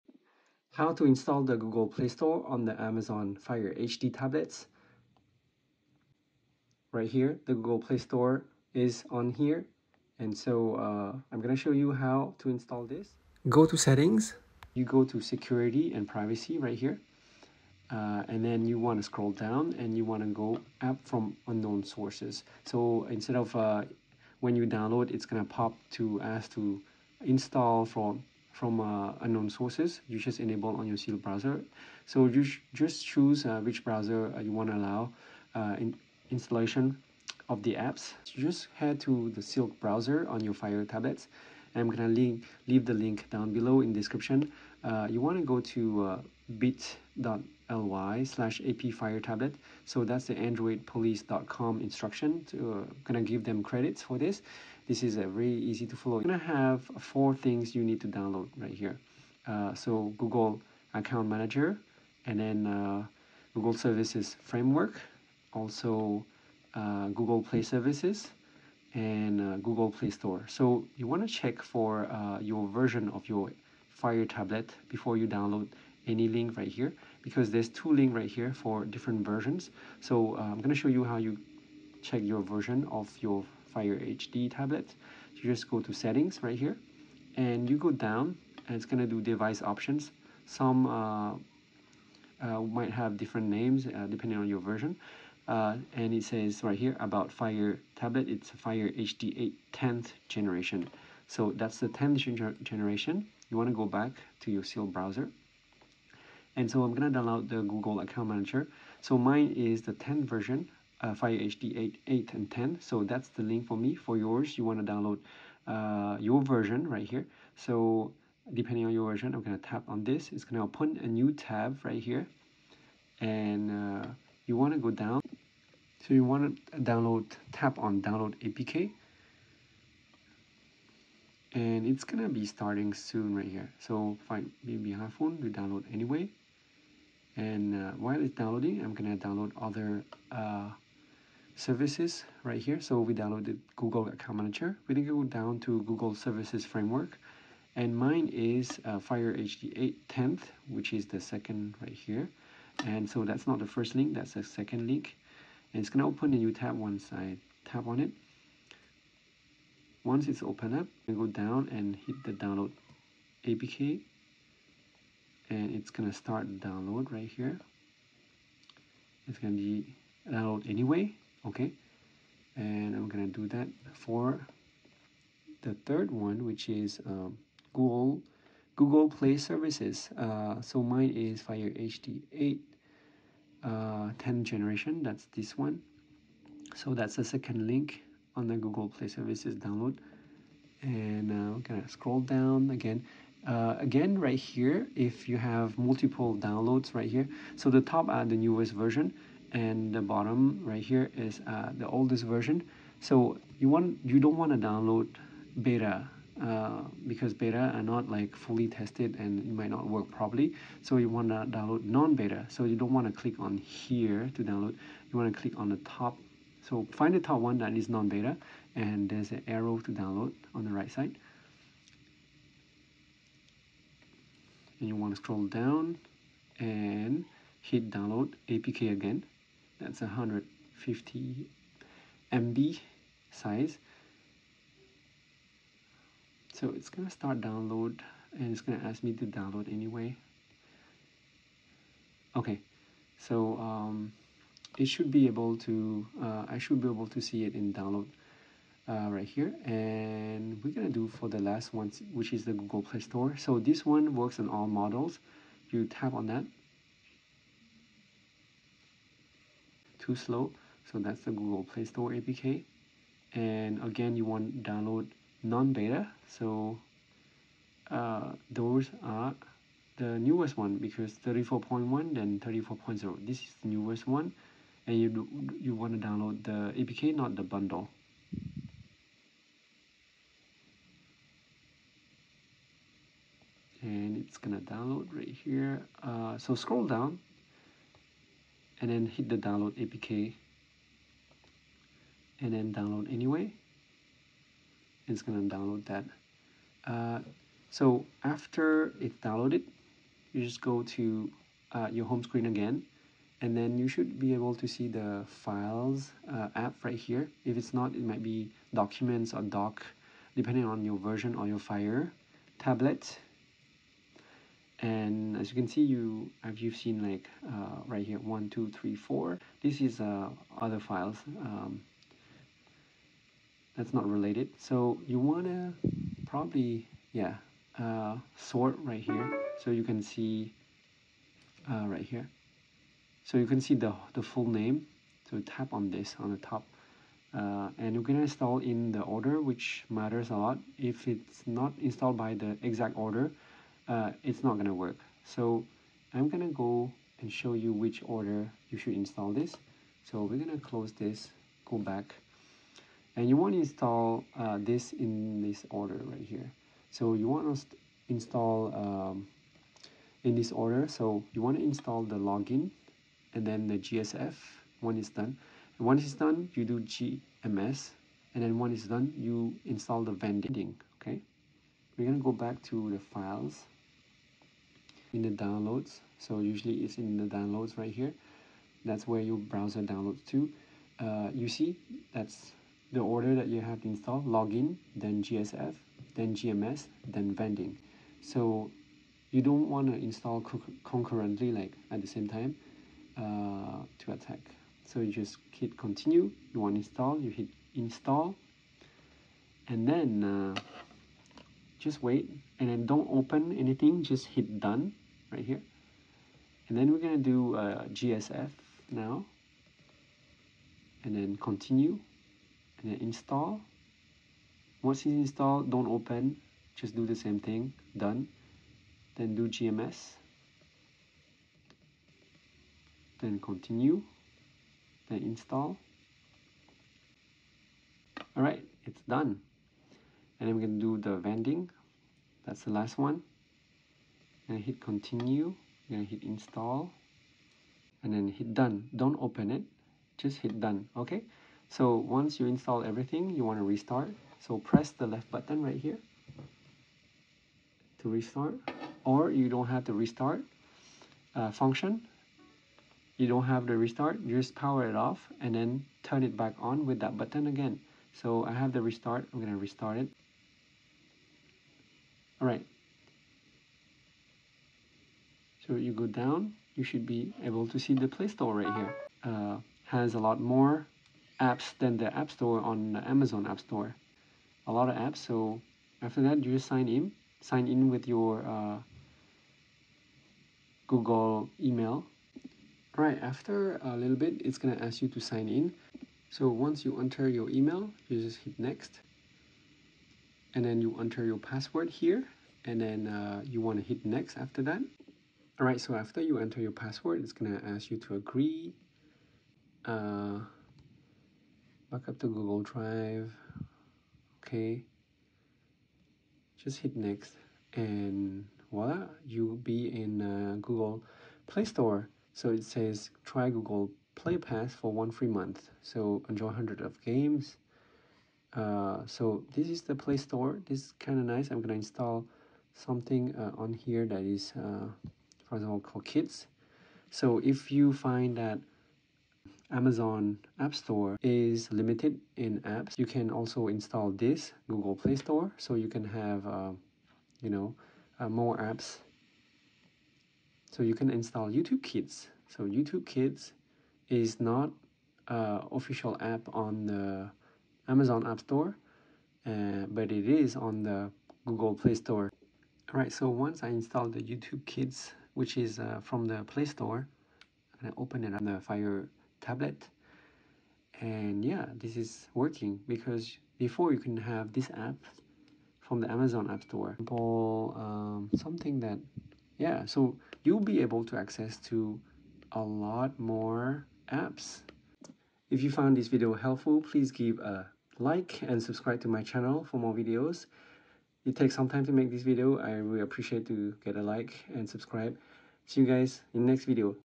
How to Download Google Play Store on Any Fire Tablets (Step by Step Tutorial) | NO PC Required
In this video, I’m going to show you how to install the Google play store on your Amazon fire tablets without a computer.